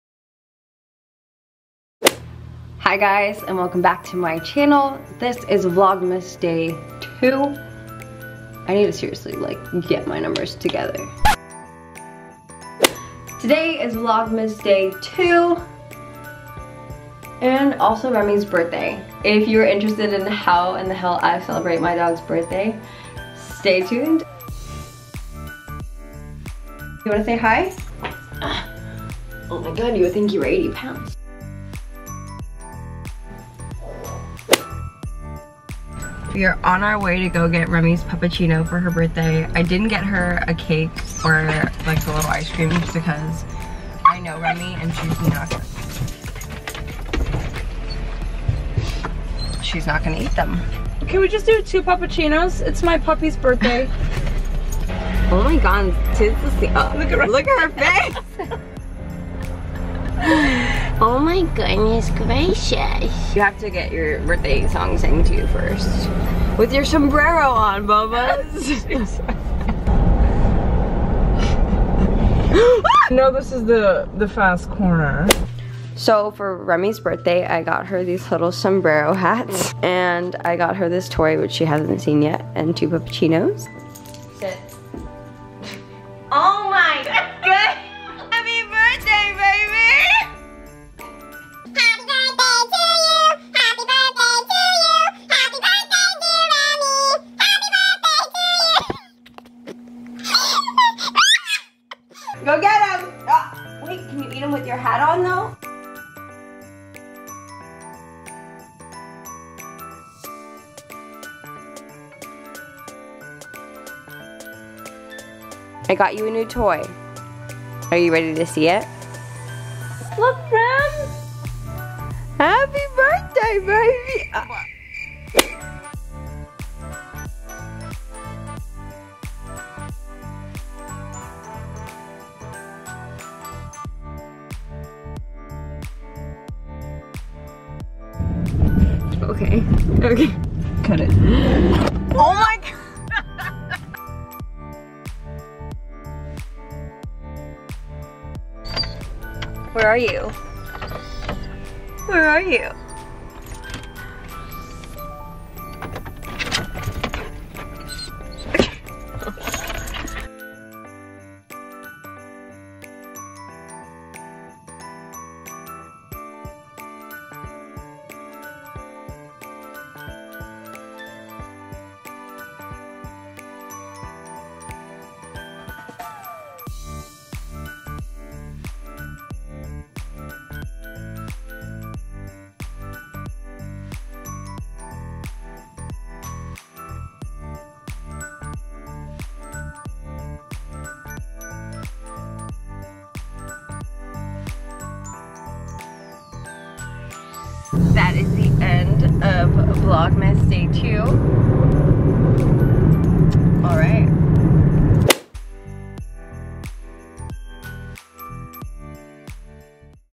VLOGMAS || REMIS BIRTHDAY!!!!!!!
Hi everyone, meet my pup Remi, she's got an incredible personality like her mother LOL!

ENJOY VLOGMAS DAY 2!